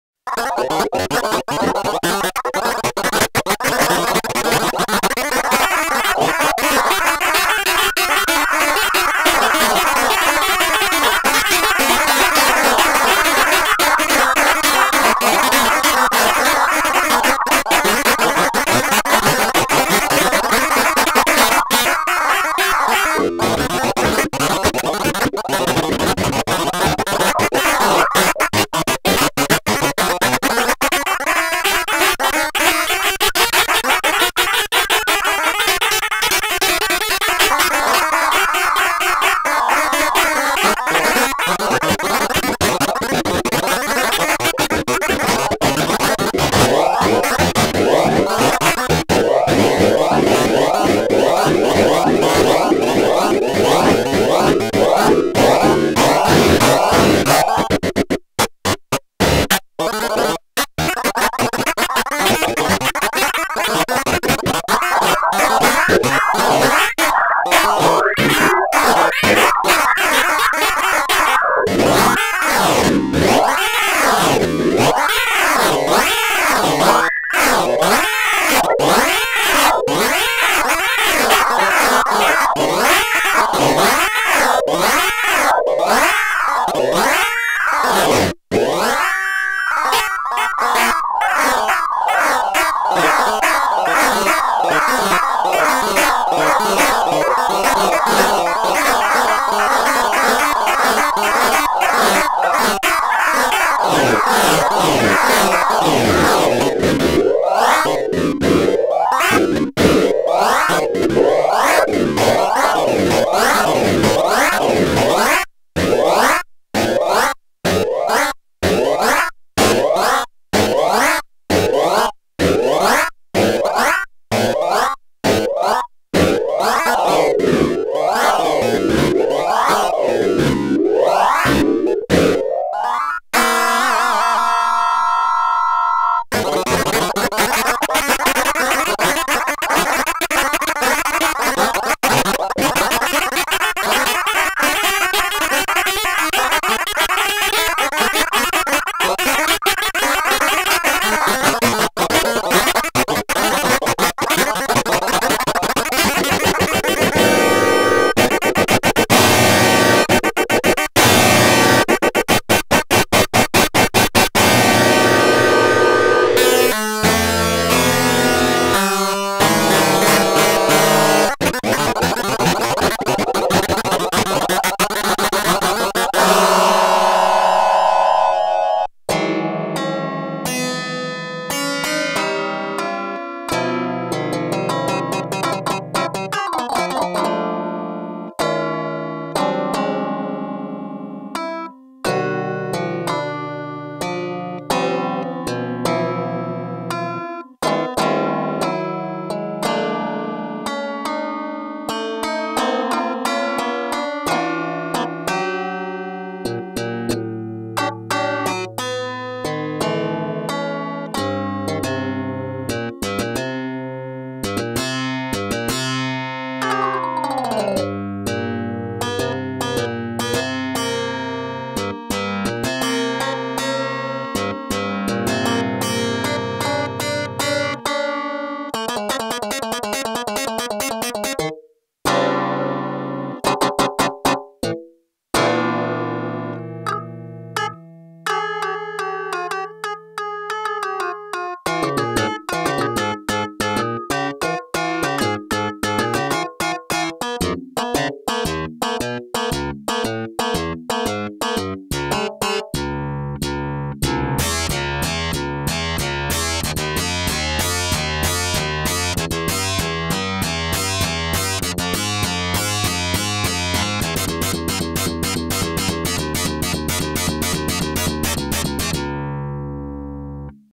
Microtonal Entities
for scientific real sound investigation. dx7 ii d micro-tuned to my "frequency mode" it explains the origins of animal songs (ie frogs/birds/etc)
this is the base level of microtonalism in the world now
meaning there is no further research in the area of non-temperament microtonal tunings based on frequency (see Leibniz/Huygens/Fokker holy trinity of science and sonic philosophy)